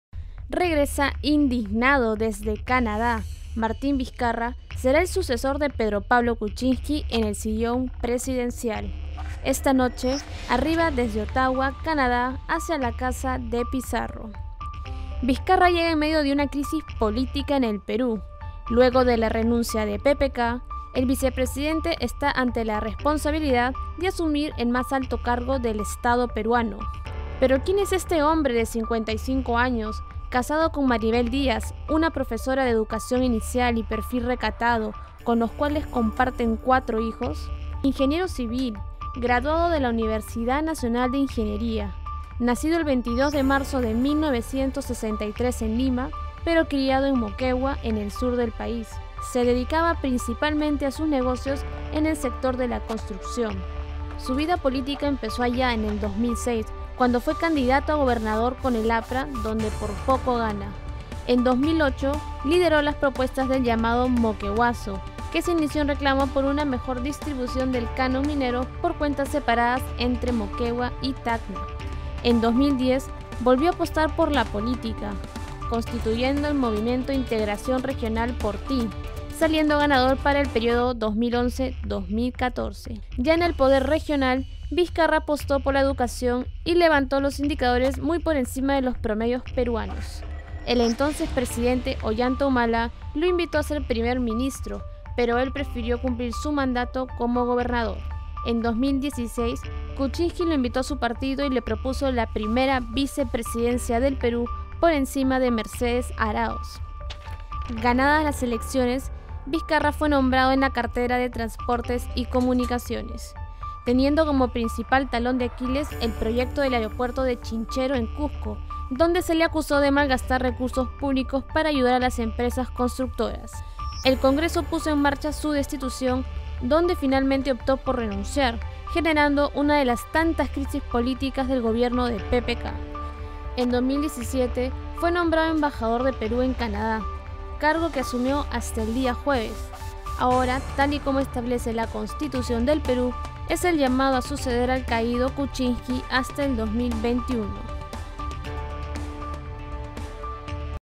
PERFIL MARTÍN VIZCARRA
CONOZCAMOS UN POCO MÁS DEL PERFIL DE MARTÍN VIZCARRA, QUIEN EL DÍA DE MAÑANA ESTARÁ ASUMIENDO COMO PRESIDENTE DEL PERÚ.